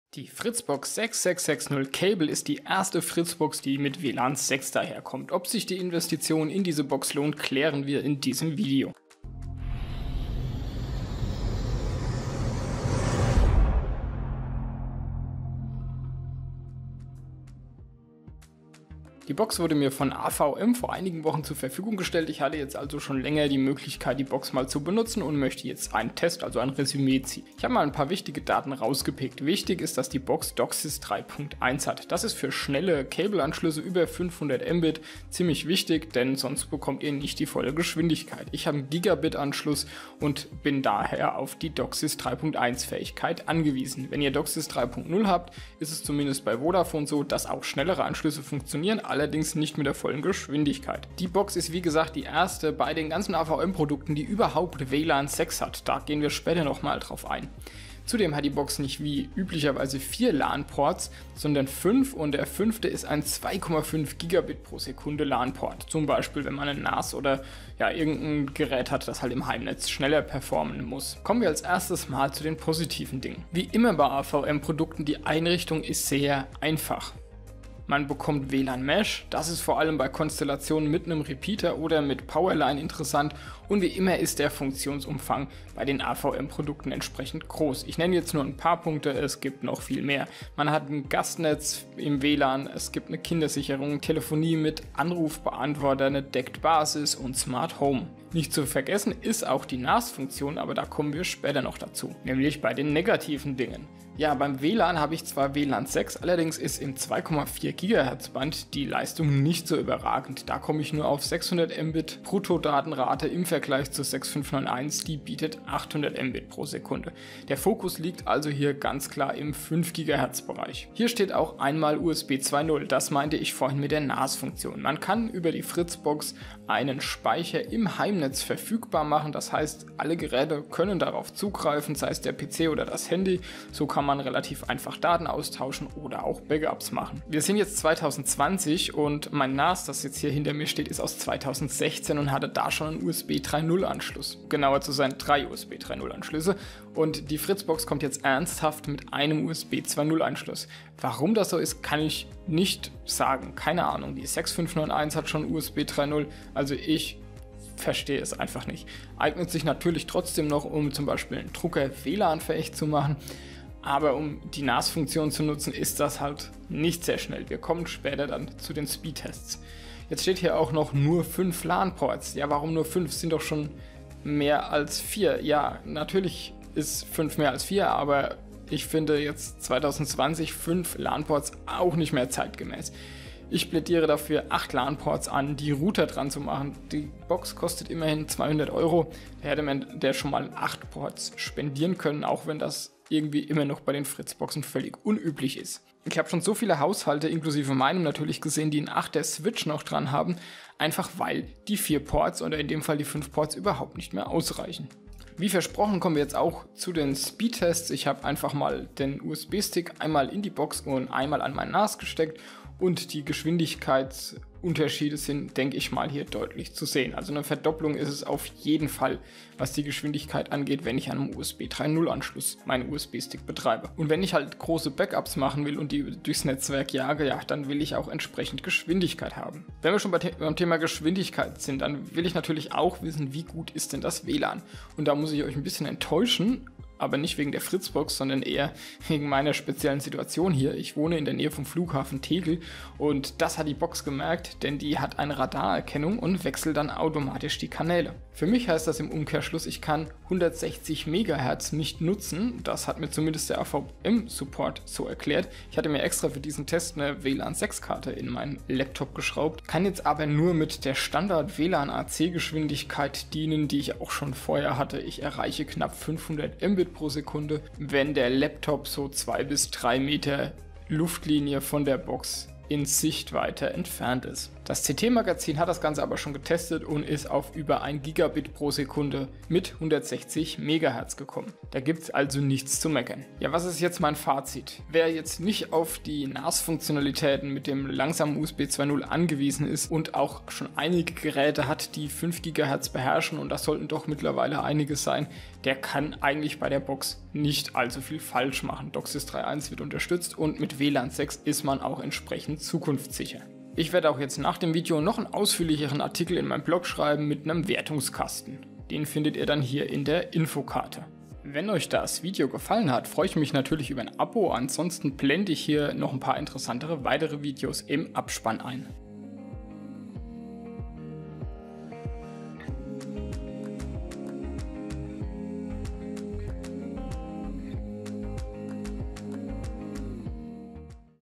Fritz!Box 6660 Cable mit WLAN 6 AX im Test - das leistet die neue DOCSIS 3.1 FritzBox im Speedtest
Aktueller Preis (Werbung)
Die erste FritzBox mit WLAN AX ist überraschenderweise eine Cable Box für Kabel und nicht für DSL Anschlüsse. Ob die Box trotz USB 2.0 im Test überzeugen konnte zeigt dieses Video.